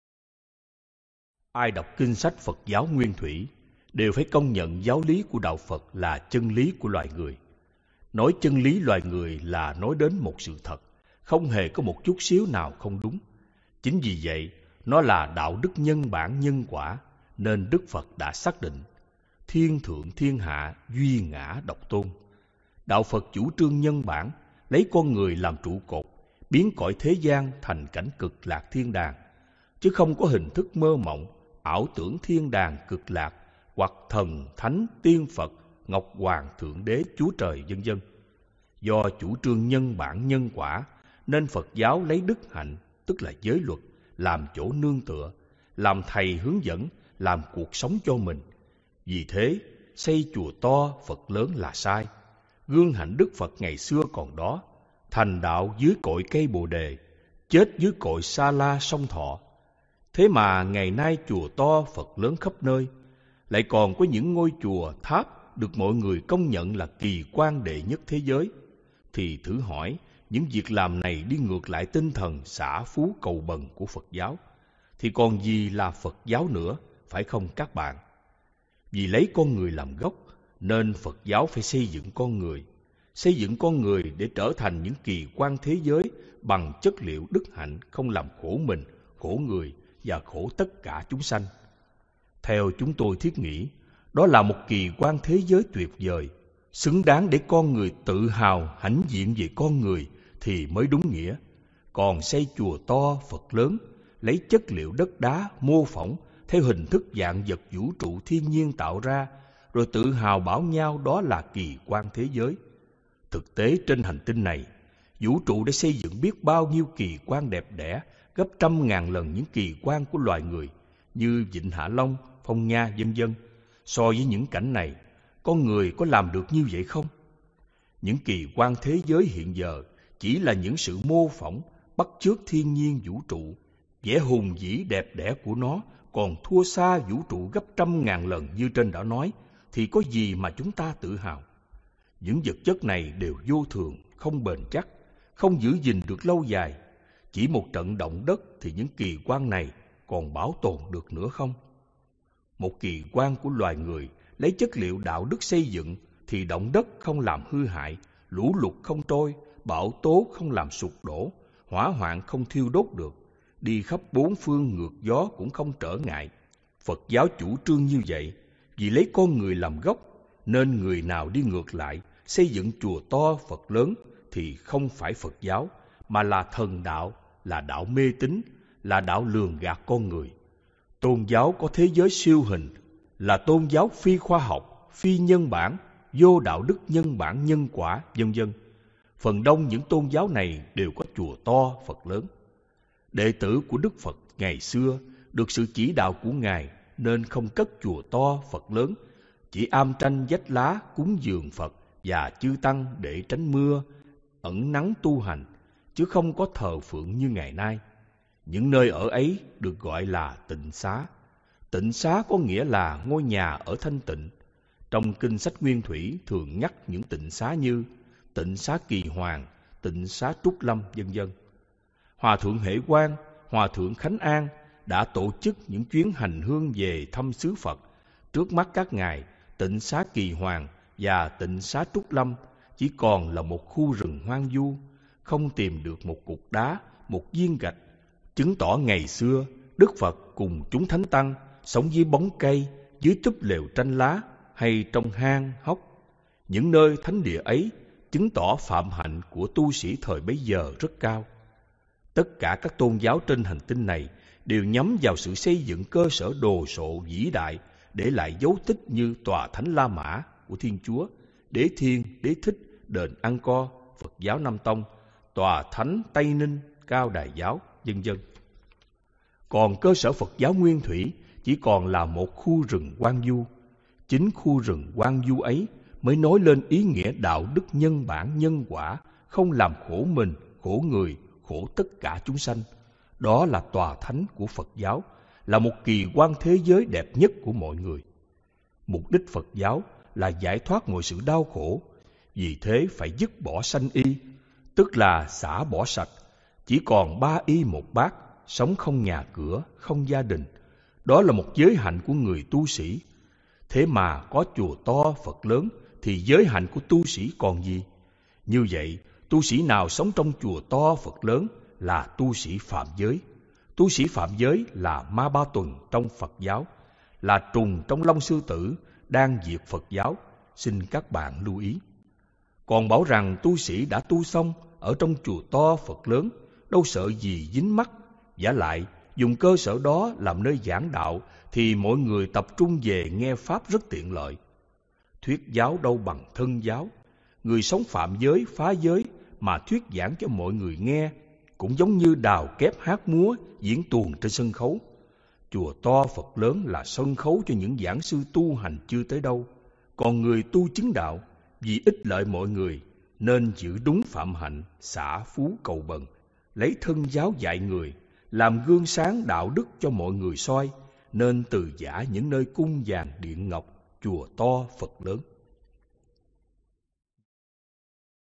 Xây chùa to phật lớn là đi ngược giáo pháp Đạo Phật! Alahan Thông Lạc giảng.
Trích lời giảng của TL Thông Lạc, trong kinh sách Tu Viện Chơ n Như.